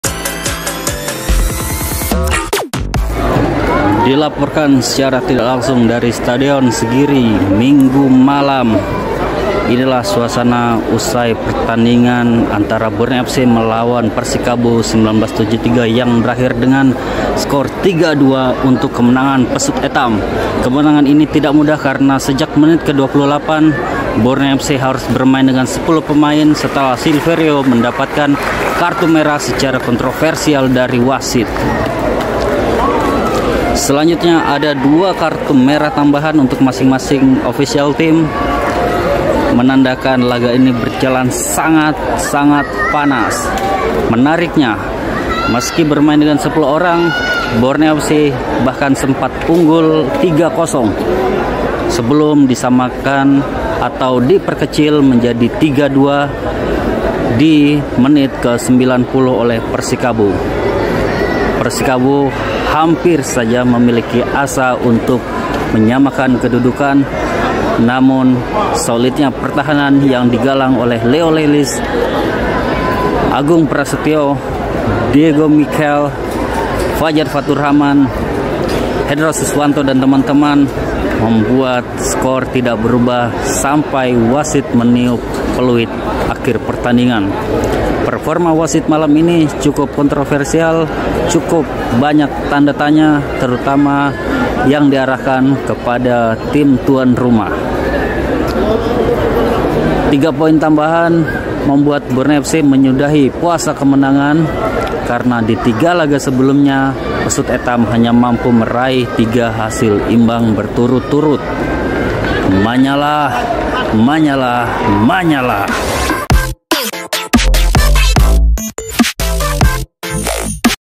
Borneo FC Taklukkan Persikabo dengan 10 Pemain | Kaltim Faktual
Borneo Fans, gimana mentalitas pemain Borneo FC di laga ini?